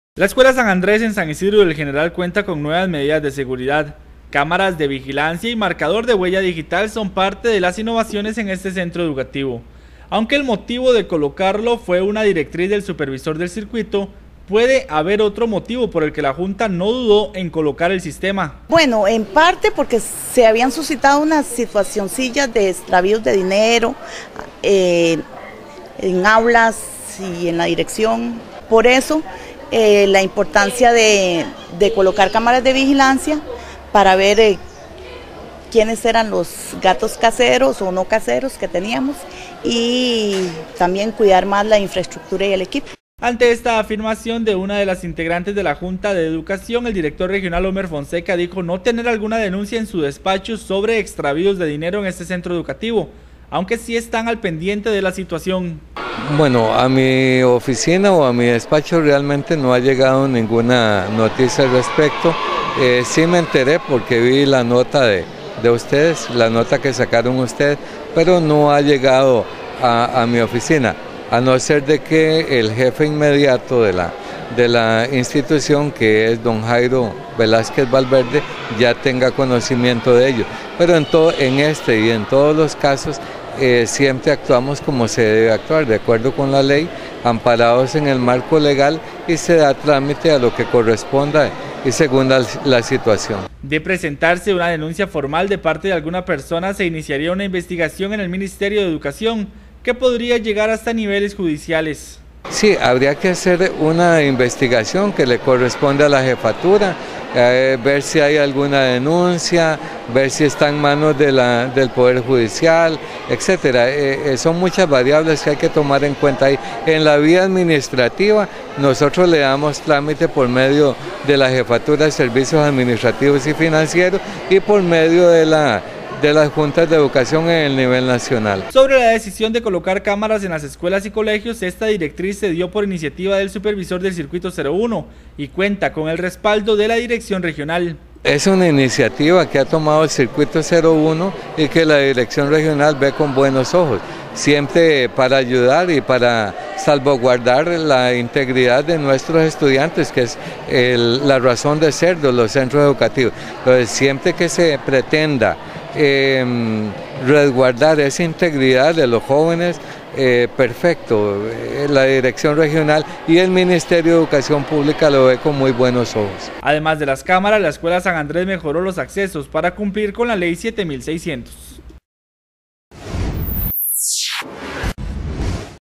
Director Regional de Educación dice no tener denuncias por extravíos en la Escuela San Andrés
La escuela San Andrés en San Isidro de El General cuenta con nuevas medidas de seguridad, cámaras de vigilancia y marcador de huella digital son parte de las innovaciones en este centro educativo, aunque el motivo de colocarlo fue una directriz del supervisor del circuito, puede haber otro motivo por el que la Junta no dudó en colocar el sistema.